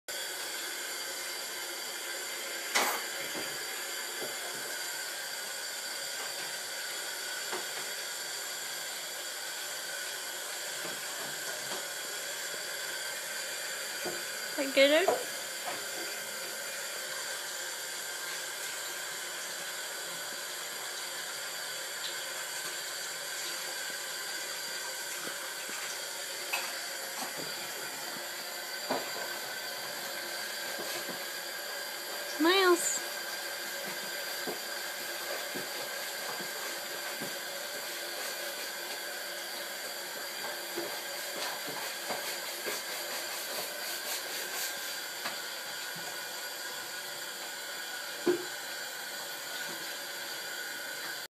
Miles and Otis lip lickin
I gave our cats wet food and they wouldn't stop licking their chops.